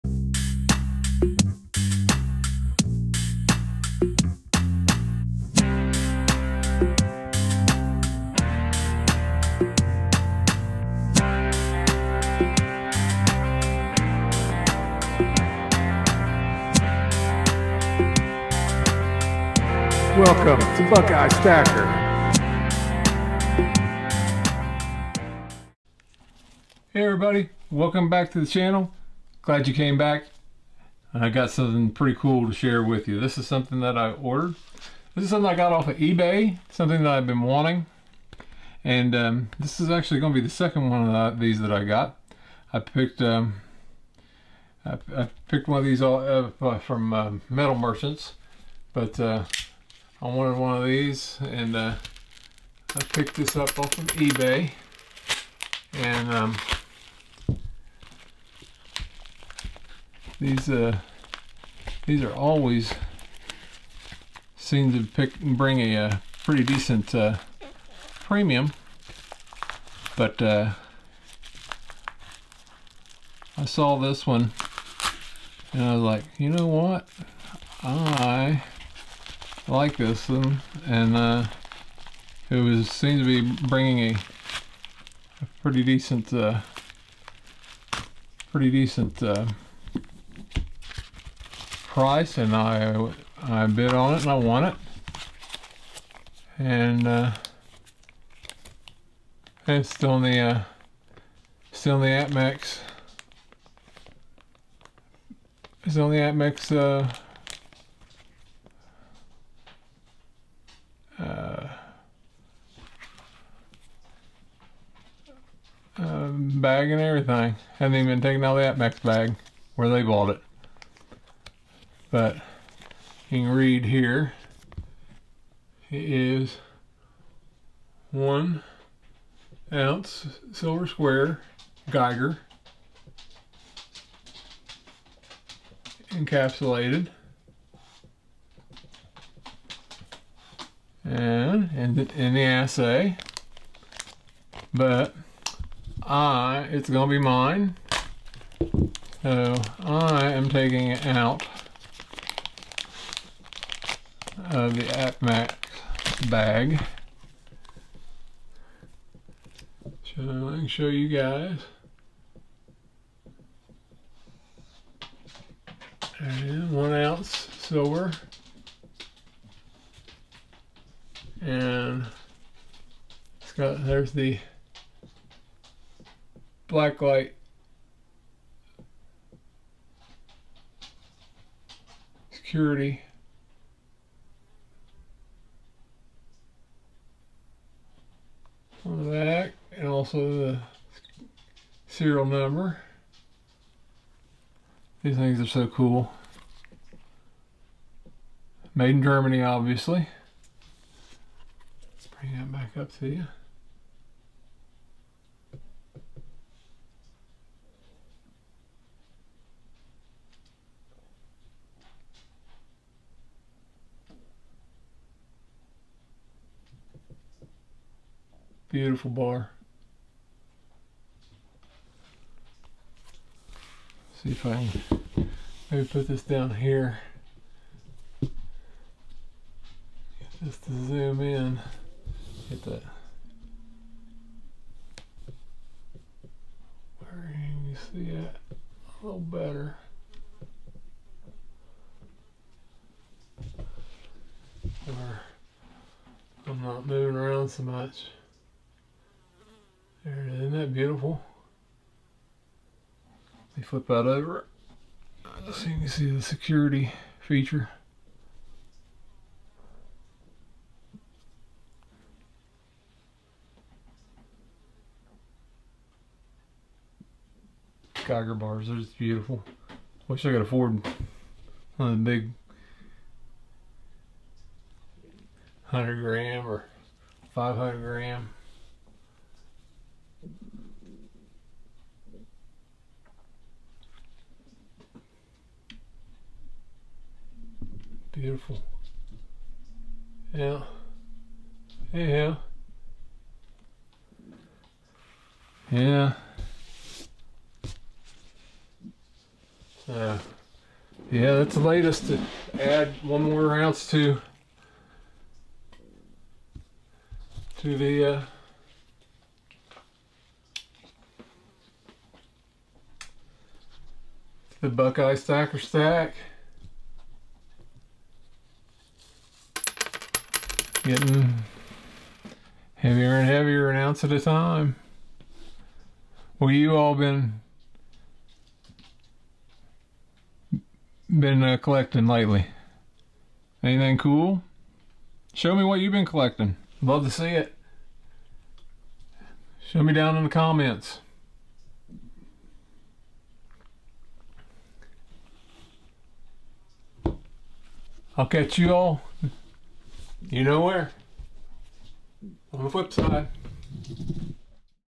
Geiger!!!! Buckeye Stacker #17 silverstacker silver # preciousmetals
I obtained another 1 ounce Geiger from eBay.  These are absolutely beautiful!!!

- Canon SL2 DSLR Camera
- Shure SM7B Mic
- Rode PSA 1 Boom Stand
- Yamaha AG03 Audio Mixer
- Audio Technica ATH-M20X Headphones